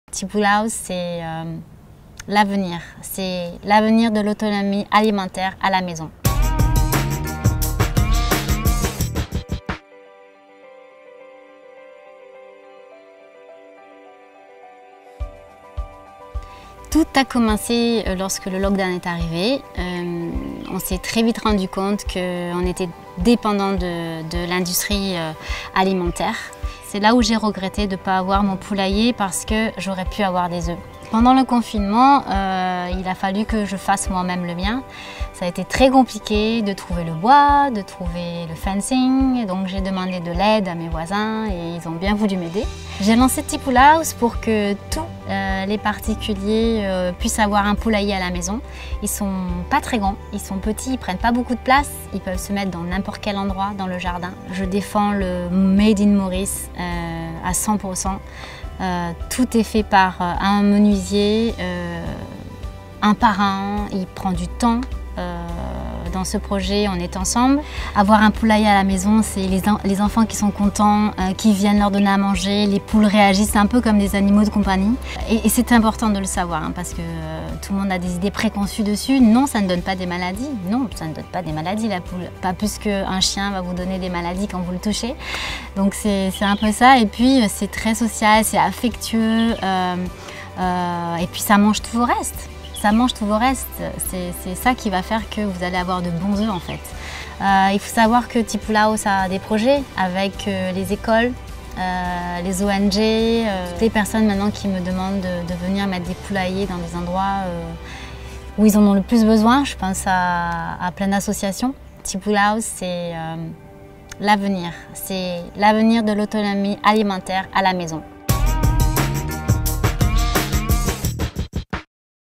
Vers l’autonomie alimentaire
TIpoule House, c’est avoir son propre poulailler à la maison et se lancer ainsi vers l’autonomie alimentaire. Catherine, la fondatrice de ce projet raconte ses inspirations, son projet et son offre aux Mauriciens.